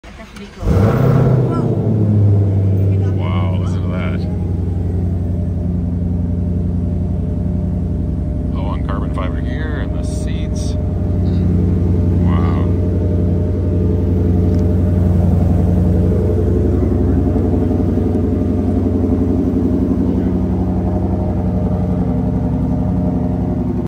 Hertz Shelby GT500-H 900 HP (1 of only 18) Las Vegas Airport. Stay tuned video tour coming...
Hertz Shelby GT500 with 900 horsepower available for rent at a handful of airport locations.  There are 6 cars in white and 12 in black.  This white one is available for rent at Las Vegas International airport.